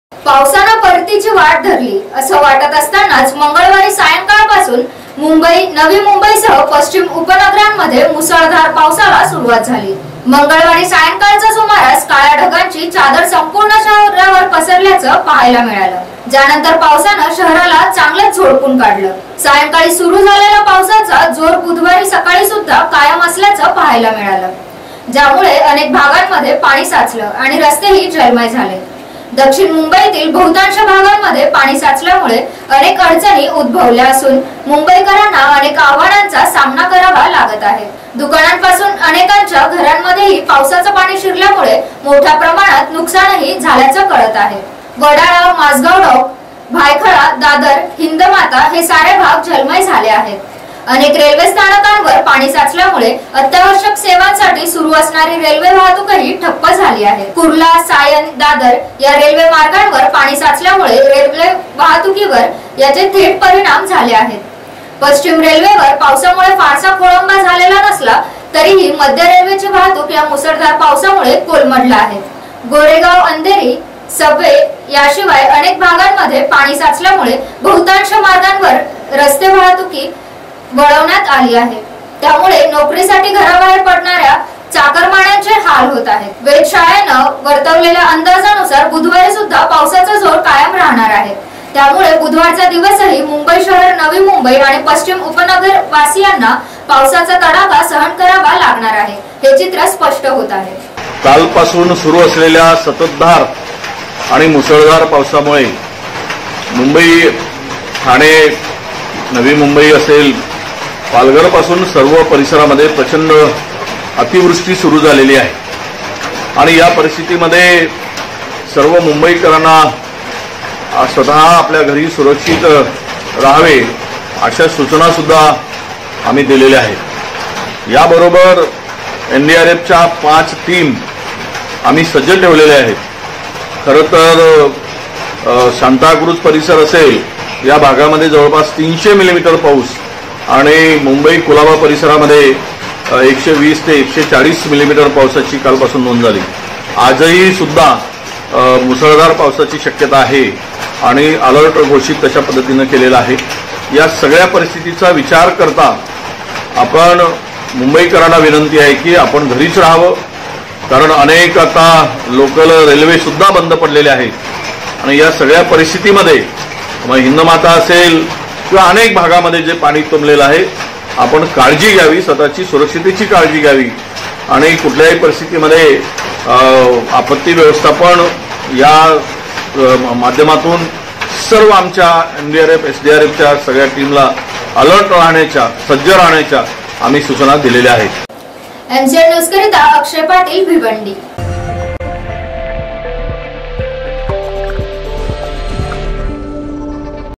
रस्ते जलमय; मुंबईत रात्रभर पावसाची जोरदार बॅटिंग
MCNNEWSJALGAONJAMOD. मुंबई : पावसानं परतीची वाट धरली असं वाटत असतानाच मंगळवारी सायंकाळपासून Mumbai मुंबई, नवी मुंबईसह पश्चिम उपनगरामध्ये मुसळधार पावसाला सुरुवात झाली. मंगळवारी सायंकाळच्या सुमारास काळ्या ढगांची चादर संपूर्ण शहरावर पसरल्याचं पाहायला मिळालं. ज्यानंतर पावसानं शहराला चांगलच झोडपून काढलं.

सायंकाळी सुरु झालेल्या पावसाचा जोर बुधवारी सकाळीसुद्धा कायम असल्याचं पाहायला मिळालं. ज्यामुळं अनेक भागांमध्ये पाणी साचलं आणि रस्तेही जलमय झाले. दक्षिण मुंबईतील बहुतांश भागांमध्ये पाणी साचल्यामुळं अनेक अडचणी उदभवल्या असून, मुंबईकरांना अनेक आव्हानांचा सामना करावा लागत आहे. दुकानांपासून अनेकांच्या घरांमध्येही पावसाचं पाणी शिरल्यामुळं मोठ्या प्रमाणात नुकसानही झाल्याचं कळत आहे.

वडाळा, माझगाव डॉक, भायखळा, दादर, हिंदमाता हे सारे भाग जलमय झाले आहेत. अनेक रेल्वे स्थानकांवर पाणी साचल्यामुळं अत्यावश्यक सेवांसाठी सुरु असणारी रेल्वे वाहतूकही ठप्प झाली आहे. कुर्ला, सायन, दादर या रेल्वे मार्गांवर पाणी साचल्यामुळं रेल्वे वाहतुकीवर याचे थेट परिणाम झाले आहेत. पश्चिम रेल्वेवर पावसामुळं फारसा खोळंबा झालेला नसला तरीही मध्य रेल्वेची वाहतूक या मुसळधार पावसामुळं कोलमडलं आहे. गोरेगाव, अंधेरी सबवे याशिवाय अनेक भागांमध्ये पणी साचल्यामुळं बहुतांश मार्गांवरी रस्ते वाहतुकही वळवण्यात आली आहे. त्यामुळं नोकरीसाठी घराबाहेर पडणाऱ्या चाकरमान्यांचे हाल होत आहेत.

वेधशाळेनं वर्तवलेल्या अंदाजानुसार बुधवारीसुद्धा पावसाचा जोर कायम राहणार आहे. त्यामुळं बुधवारचा दिवसही मुंबई शहर, नवी मुंबई आणि पश्चिम उपनगरवासियांना पावसाचा तडाखा सहन करावा लागणार आहे हे चित्र स्पष्ट होत आहे.